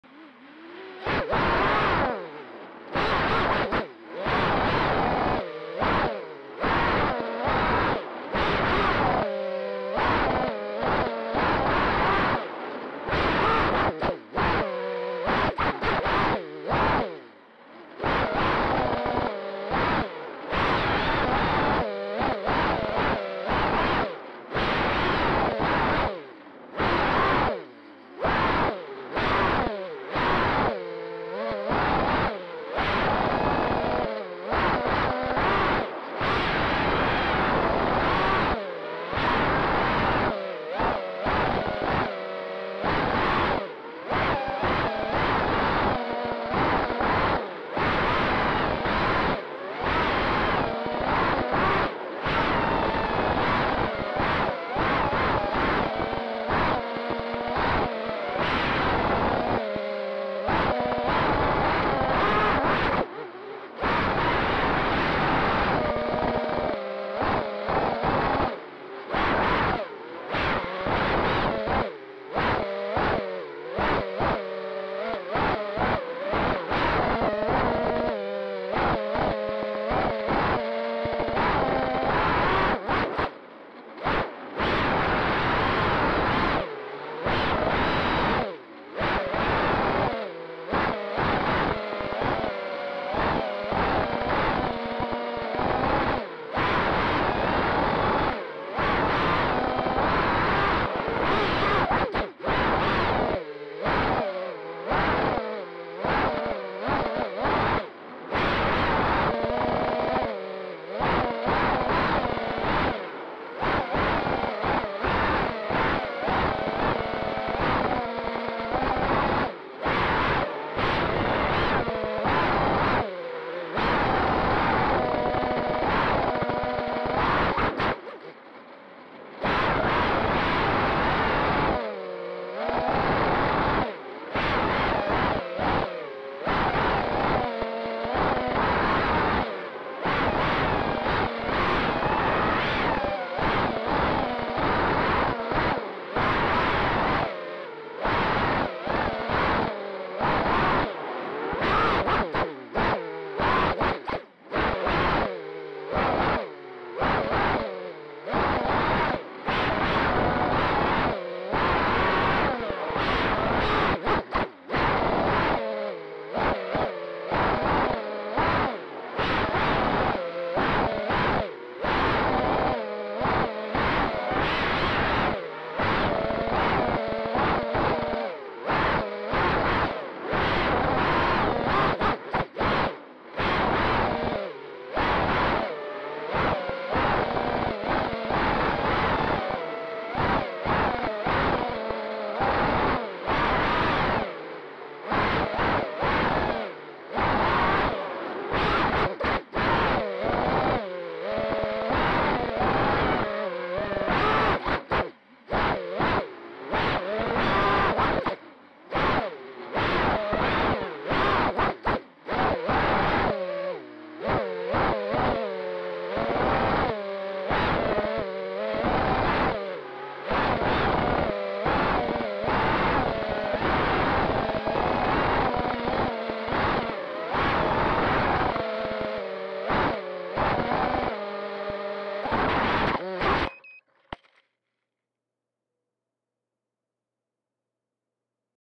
RunCam2 Test 1080p, 60fps, 12Mbps
RunCam2 setting test (RunCam2 recommend for youtube upload at 60fps)

Direct upload other than cutting front and end of video